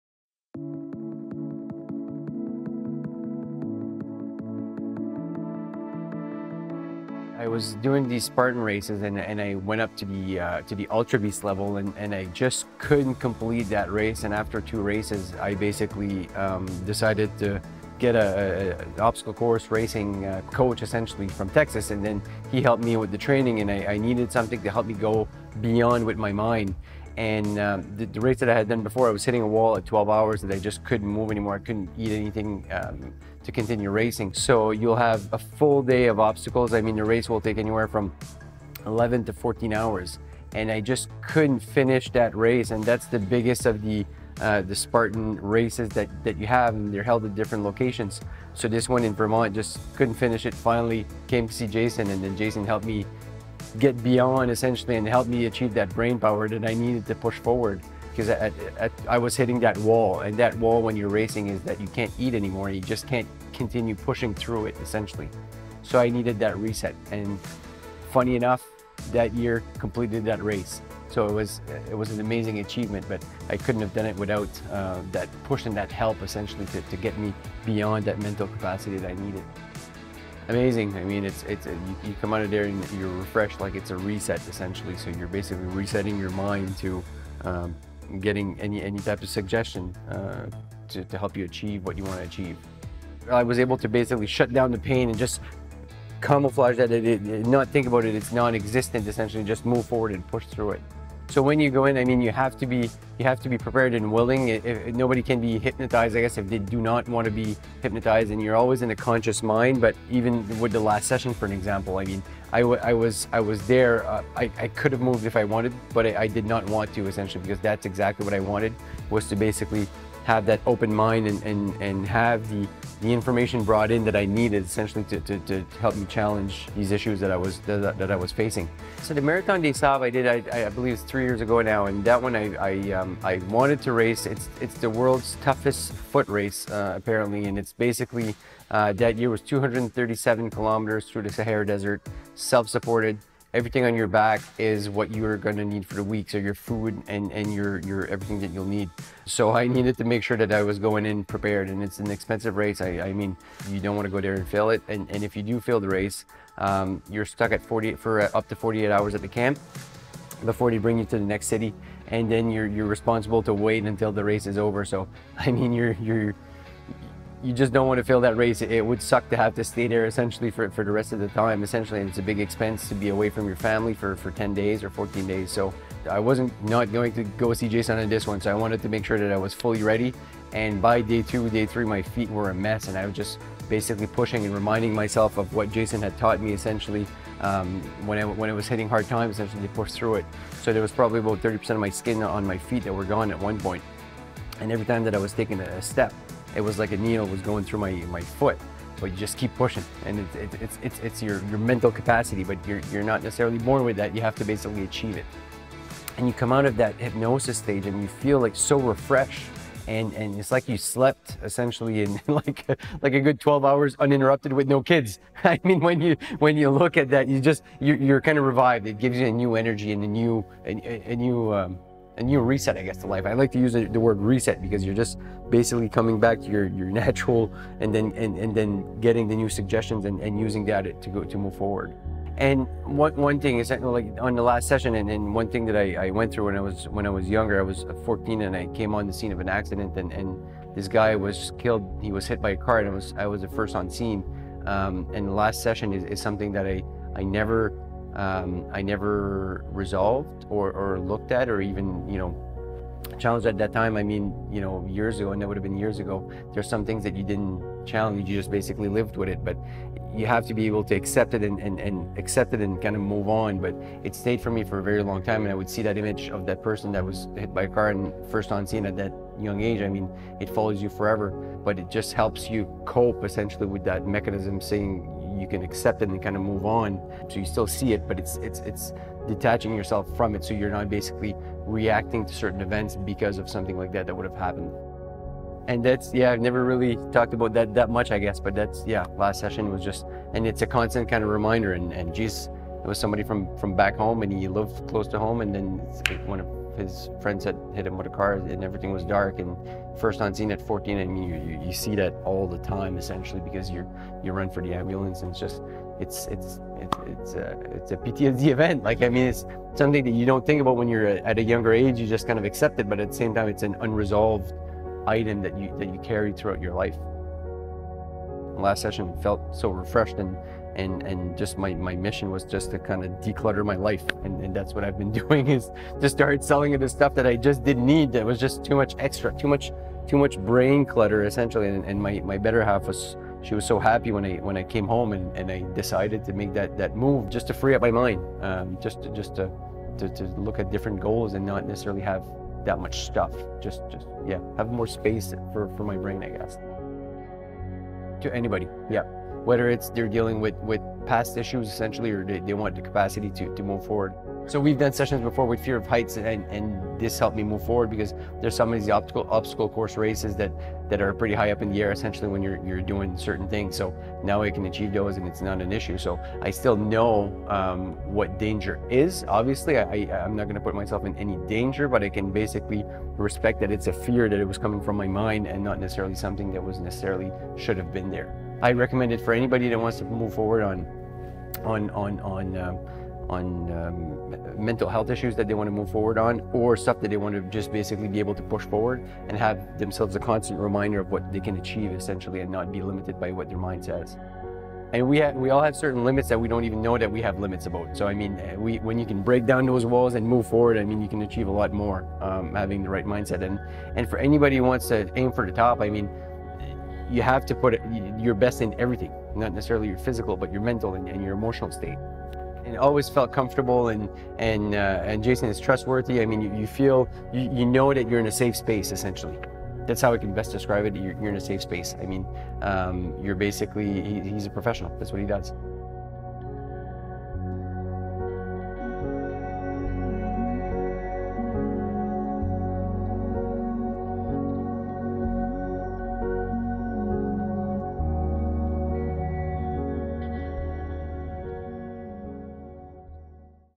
Matt “AKA – The Human Machine” (Spartan Racer & Sahara desert)
Take 7m: 32s and listen to this story. It’s truly remarkable. 🤯

⚠️Graphic Images⚠️

In this video, Mathieu talks about competing in the Spartan Ultra Beasts and was hitting a wall where he couldn’t keep on moving or eat anything.  He needed an Ultra Beast Mindset.  After working with me and completing not 1 but 3 Spartan Ultra Beasts, I worked with Matt to attack the World’s Toughest Foot Race, the Marathon Des Sables (237km in the Sahara desert - Self Supported).

He also talks about fear of heights and a traumatic event he experienced at 14 years old. He was able to let go and started to declutter his life.

I can’t thank you enough Matt for sharing your story and Hypnosis experience. Keep inspiring my friend. You are AMAZING! 🏆🥇

**I do not diagnose, prescribe or treat specific diseases. I offer my services as a complement to regular doctor's visits but not as a substitute. **

Have a great weekend everyone - Mad Love to you all - JC 💙